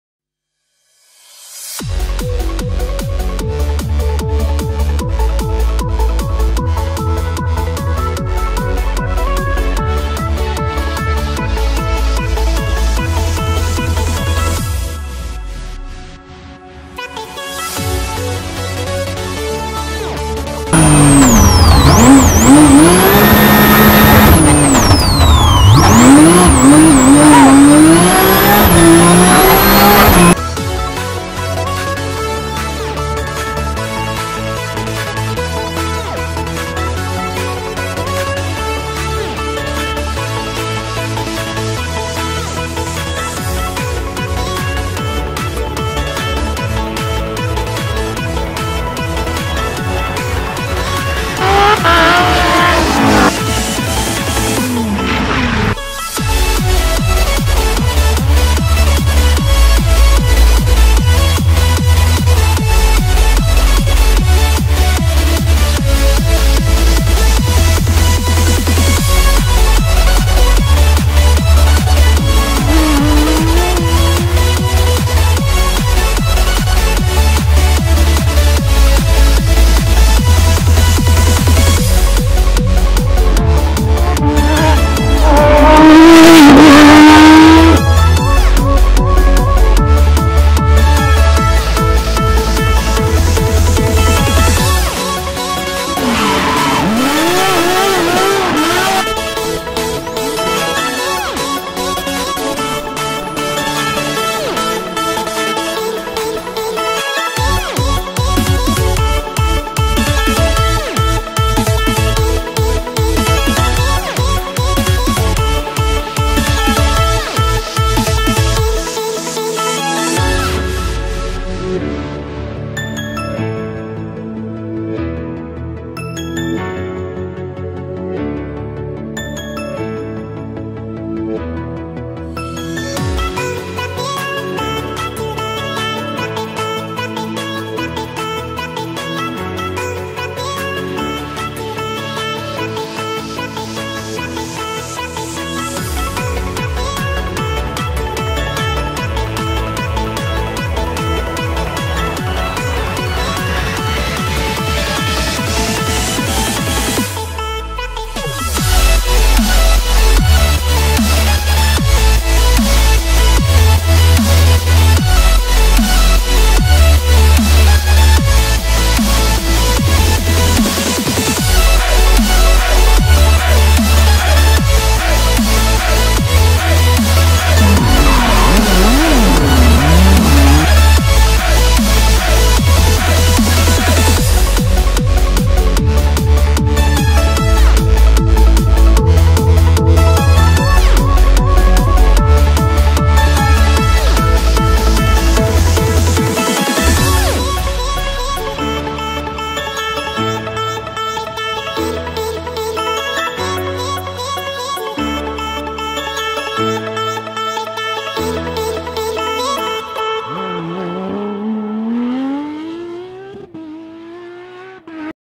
Best of 2017 Tim van Parijs Kurt Heyndrickx Porsche 997 GT3
Bekijk hier de best of 2017 van Tim van Parijs en Kurt Heyndrickx met hun Porsche 997 GT3.